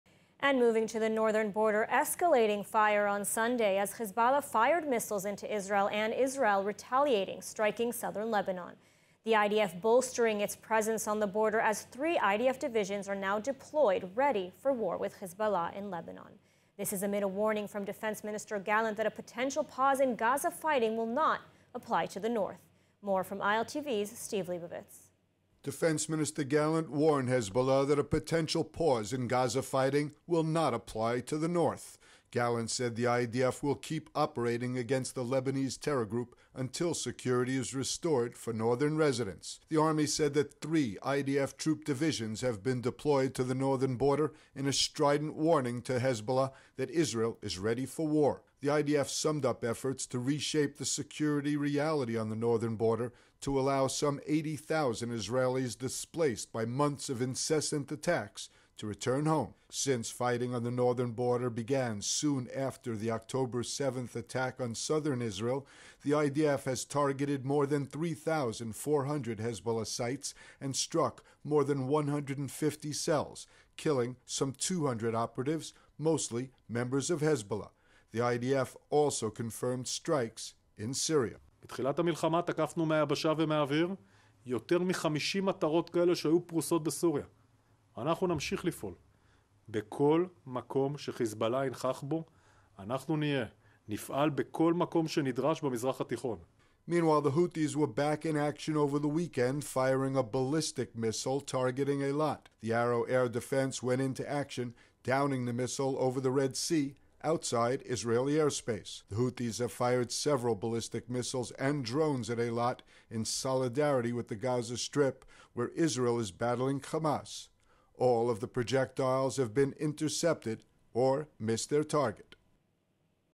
IDF prepares for war on the northern front
Defense Minister Gallant promises Hezbollah that the IDF will keep operating against the Lebanese terror group until security is restored for northern residents.